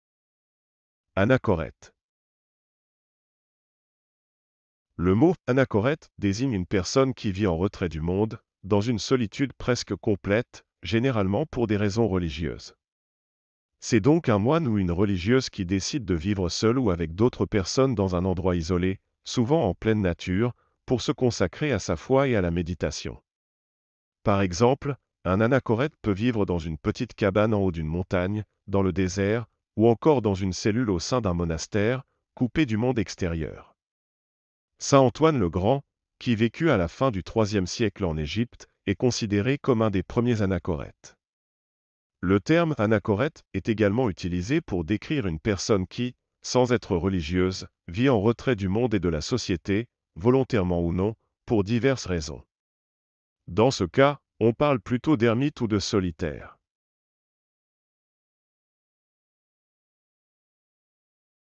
Anachorète
Le mot "anachorète" désigne une personne qui vit en retrait du monde, dans une solitude presque complète, généralement pour des raisons religieuses. C'est donc un moine ou une religieuse qui décide de vivre seul(e) ou avec d'autres personnes dans un endroit isolé, souvent en pleine nature, pour se consacrer à sa foi et à la méditation.
Par exemple, un anachorète peut vivre dans une petite cabane en haut d'une montagne, dans le désert, ou encore dans une cellule au sein d'un monastère, coupé du monde extérieur. Saint Antoine le Grand, qui vécut à la fin du IIIe siècle en Égypte, est considéré comme un des premiers anachorètes.
Le terme "anachorète" est également utilisé pour décrire une personne qui, sans être religieuse, vit en retrait du monde et de la société, volontairement ou non, pour diverses raisons. Dans ce cas, on parle plutôt d'ermite ou de solitaire.